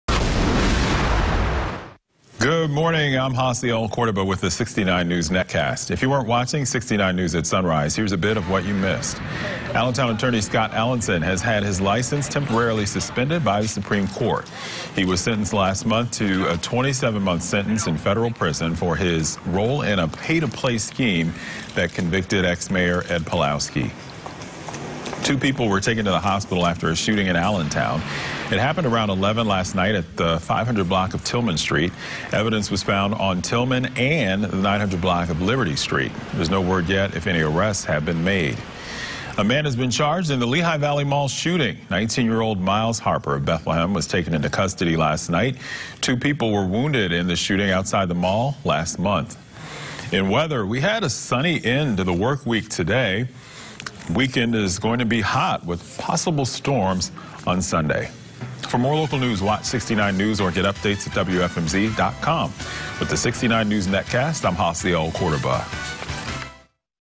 69 News morning NETCAST for 7/13/18
Here's a look at our top stories this morning: Scott Allinson's law license suspended; 2 men hospitalized after shooting in Allentown; Bethlehem man charged in Lehigh Valley Mall shooting. For more, watch 69 News or go to WFMZ.com